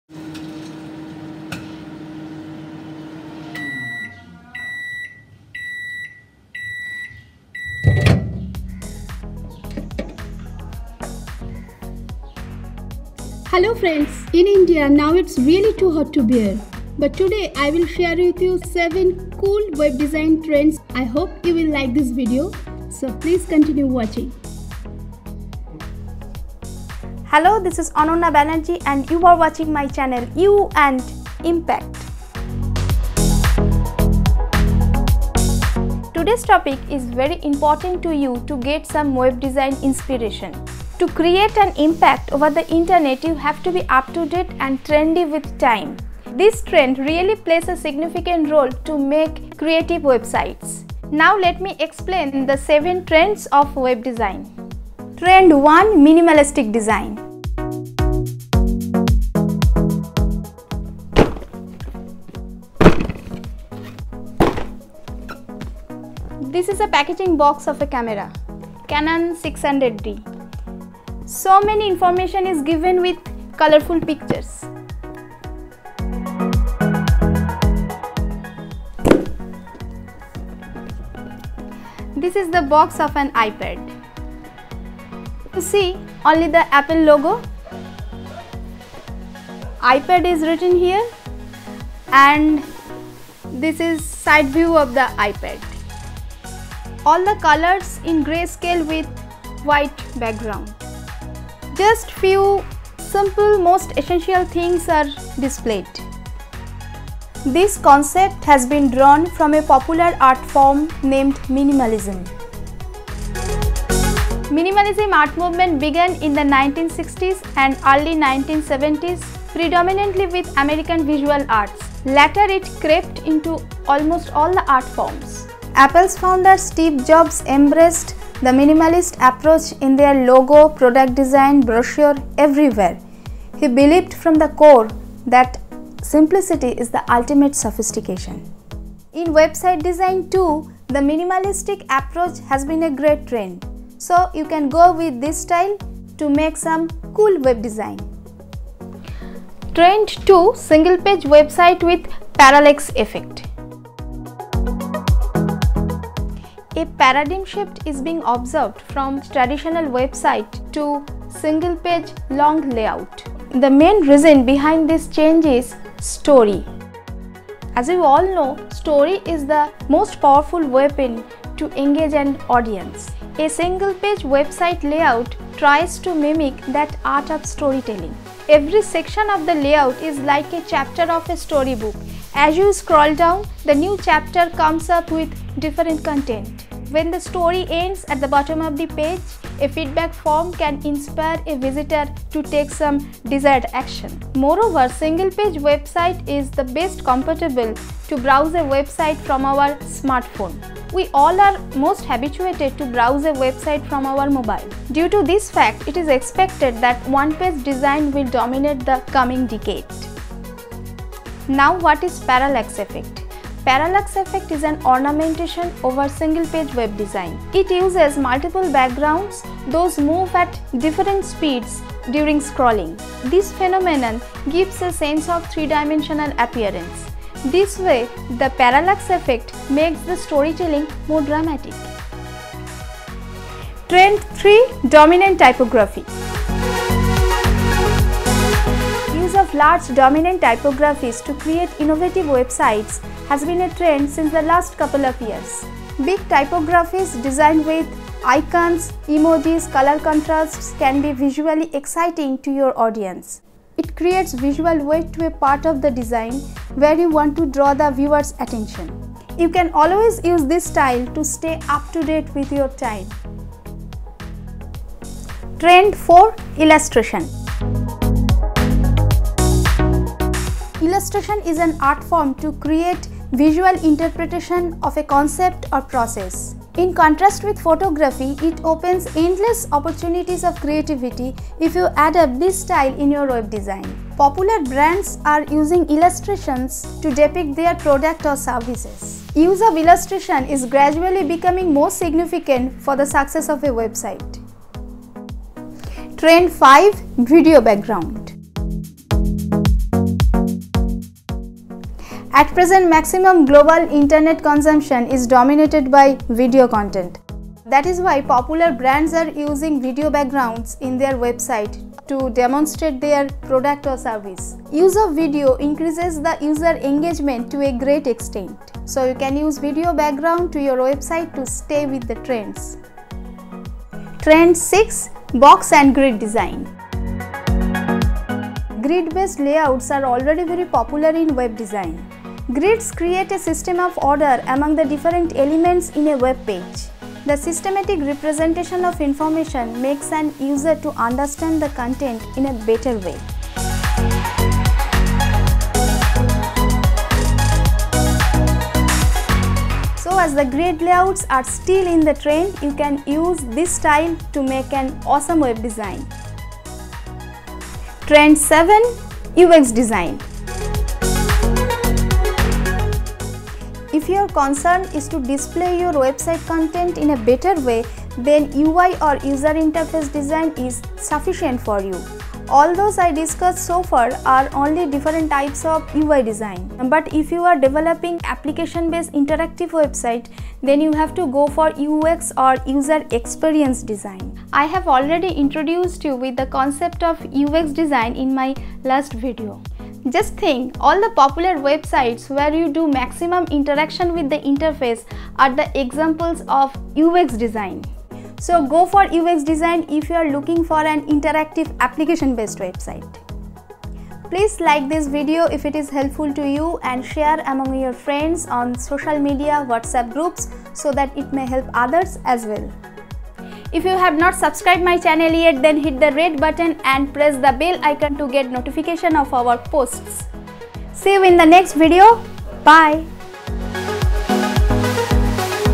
Top 7 Web design Trends in 2022 | Beautiful website design 🔥🔥🔥
Whats is the latest Website design ideas?  // Beautiful website design trends // 7 Top Web design Trends in 2022

This video is all about top design trends in 2022.  You will learn 7 most trendy web design styles. It will help you get some web design inspiration.

RELATED VIDEOS, Recommended for you to watch!

Company : Sunyam Software LLP

This video covers the following topics.